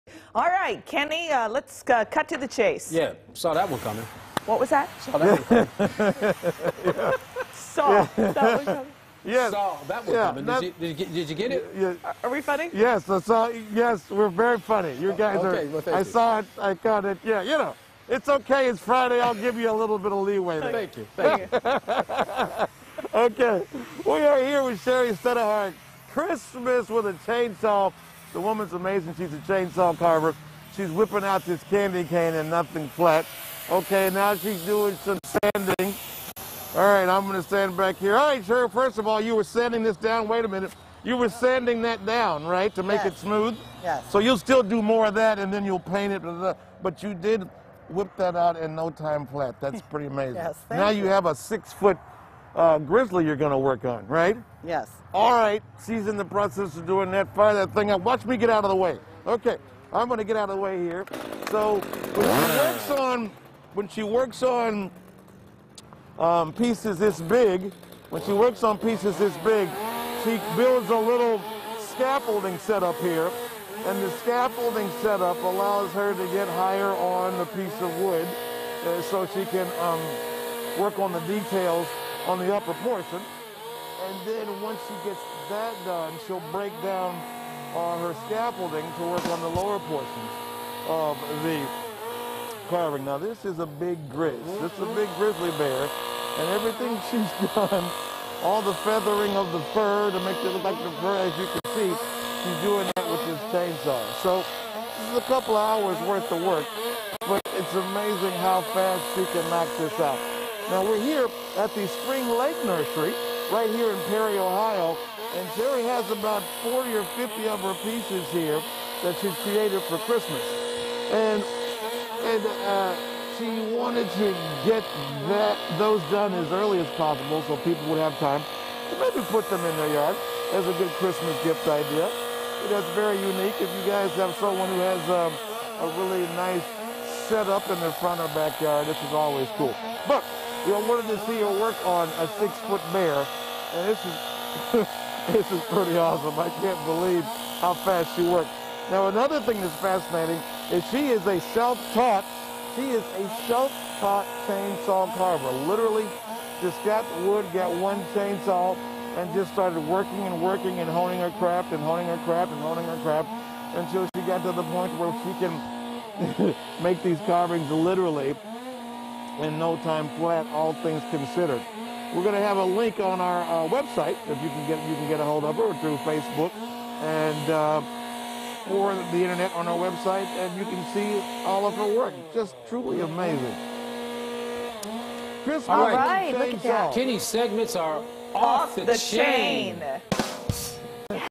Kenny's on the cutting edge with local chain saw artist
Chainsaw artist Shari Staiduhar is busy carving for Christmas. The self-taught chainsaw carver has created wooden Santa's, candy canes, and all kinds of holiday related items. Fox 8's Kenny Crumpton hung out with Shari and learned more about her art.